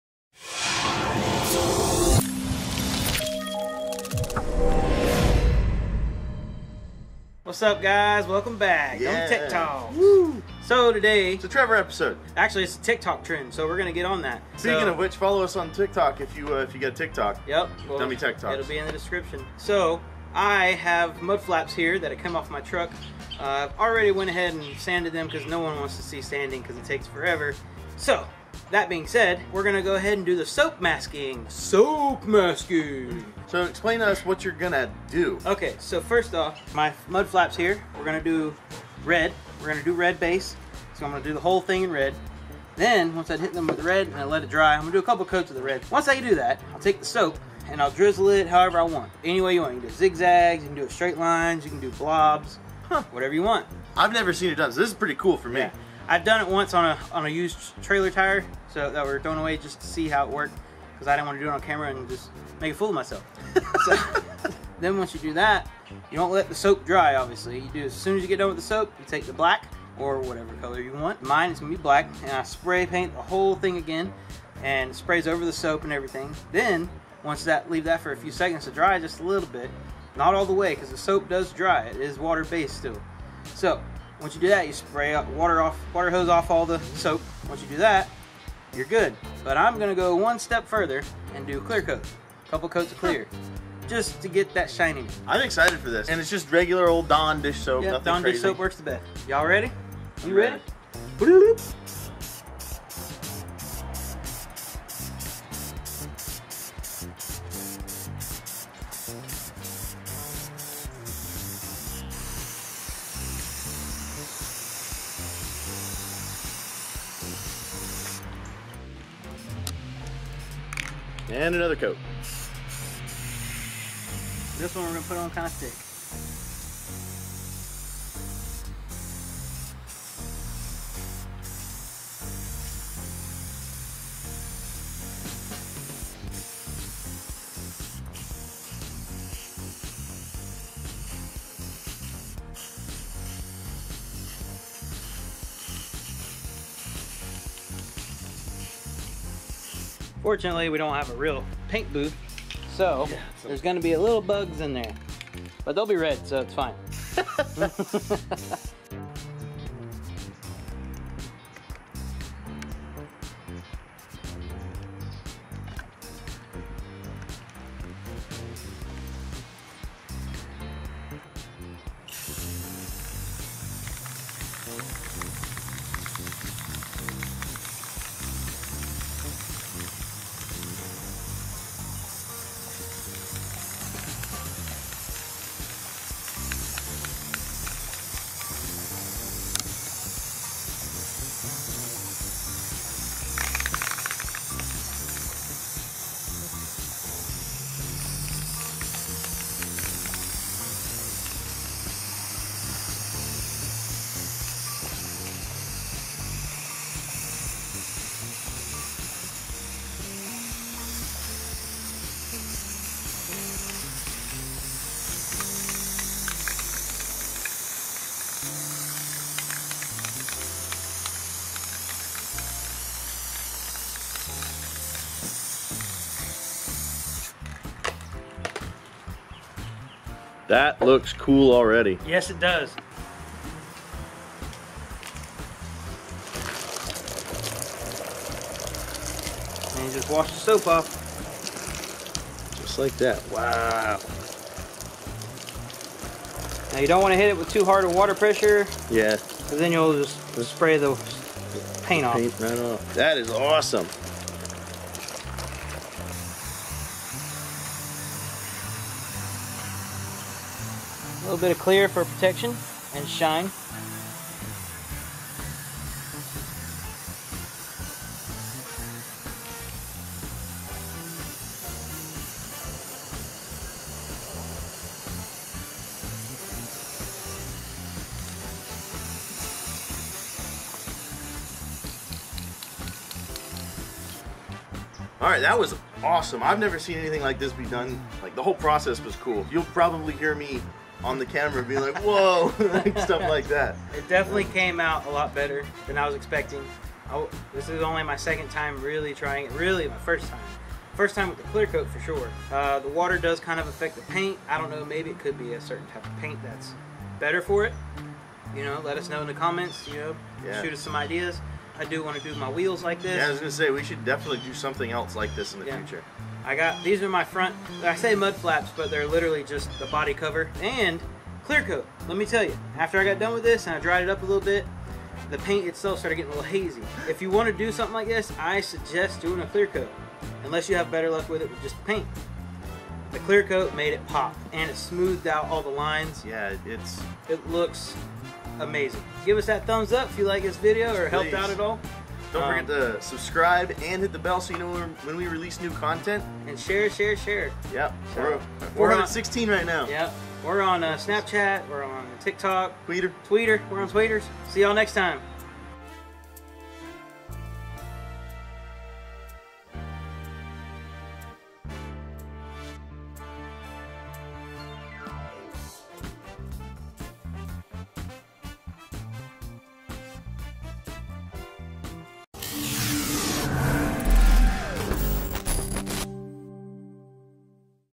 Soap Masking Trever Mud Flaps
In this video Trever uses Soap Masking to paint his stock mud flaps on his truck!

The camera made it look orange, but it did in fact turn out red and black!

Follow us on Snap chat @DummyTechTalks